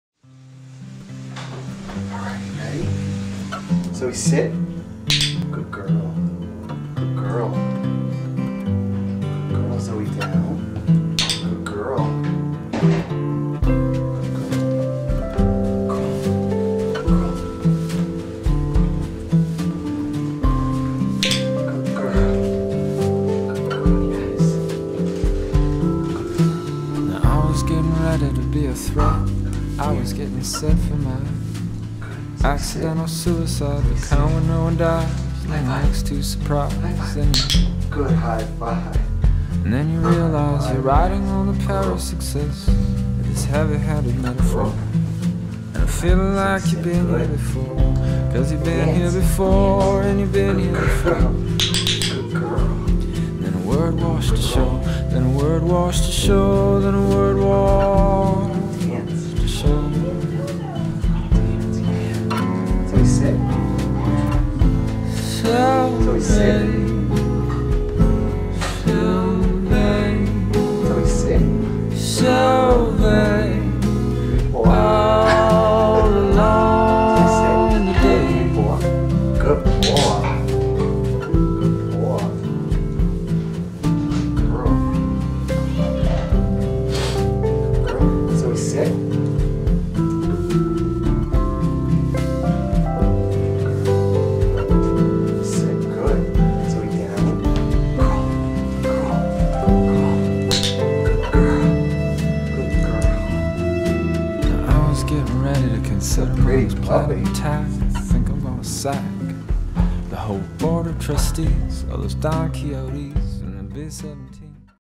zoey dog tricks
Zoey our Treeing Walker Coonhound/Boxer mix doing some fun tricks.  We saved her from a high kill shelter in west Virginia.  She's only been with us for 5 days.  She's 10 weeks old and super cute.